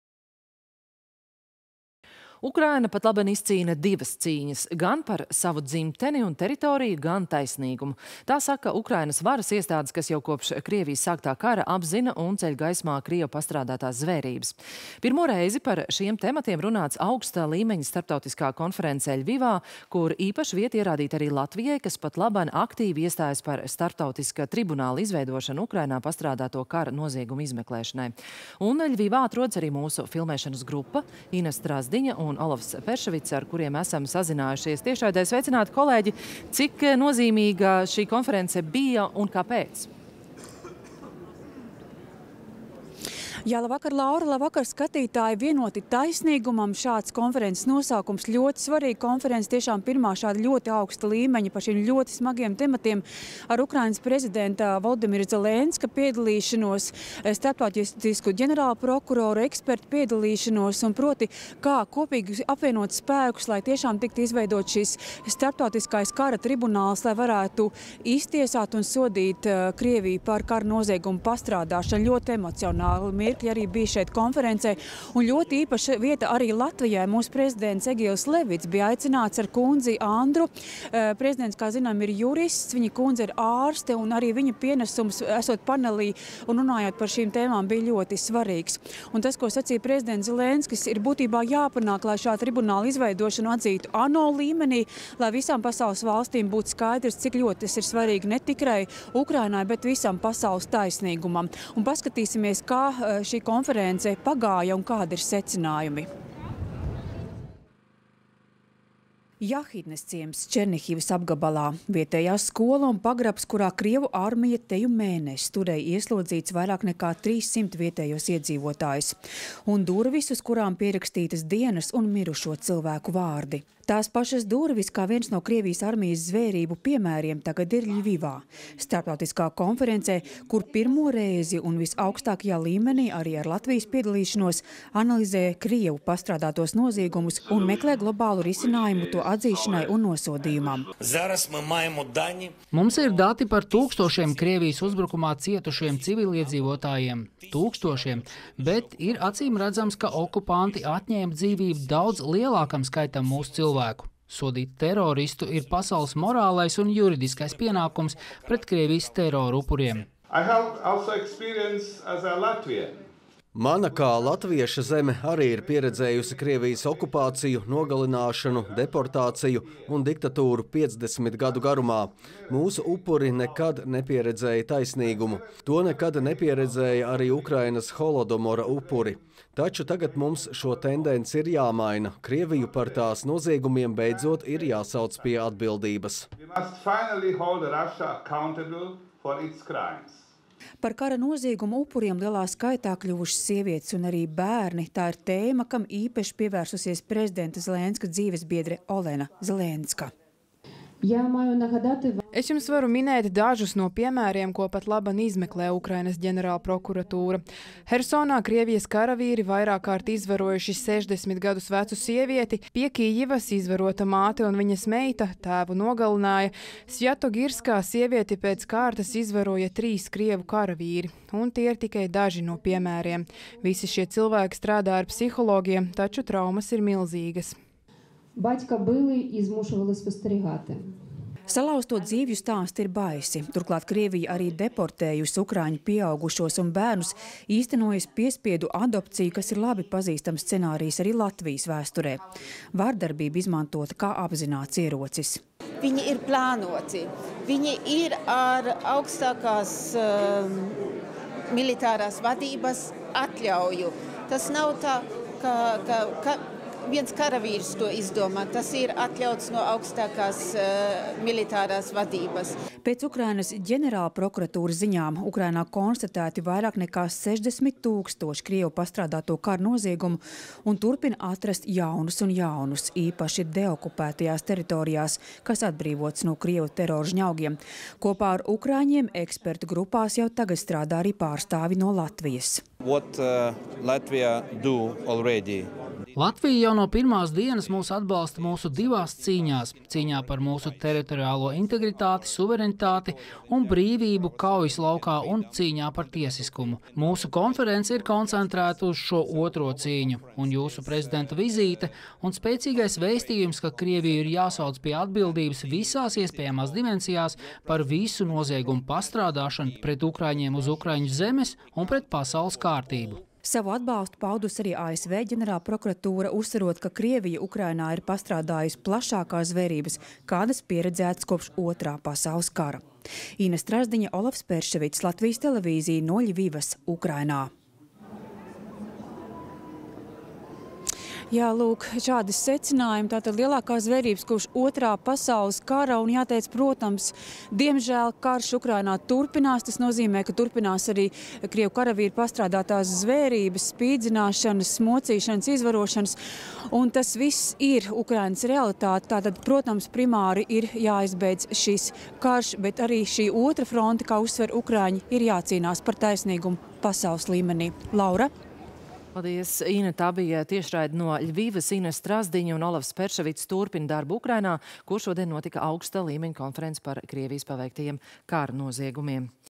Ukrainā augsta līmeņa konference par Krievijas kara noziegumiem
Ukraina patlaban izcīna divas cīņas – gan par savu dzimteni un teritoriju,  gan taisnīgumu – tā saka Ukrainas varas iestādes, kas jau kopš Krievijas sāktā kara apzina un ceļ gaismā krievu pastrādātās zvērības.
Pirmo reizi par šiem tematiem runāts augsta līmeņa starptautiskā konferencē Ļvivā, kur īpaša vieta ierādīta arī Latvijai, kas patlaban aktīvi iestājas par starptautiska tribunāla izveidošanu Ukrainā pastrādāto kara noziegumu izmeklēšanai.

Un Ļvivā atrodas arī mūsu filmēšanas grupa - Ina Strazdiņa un Olafs Peršveics, ar kuriem esam sazinājušies.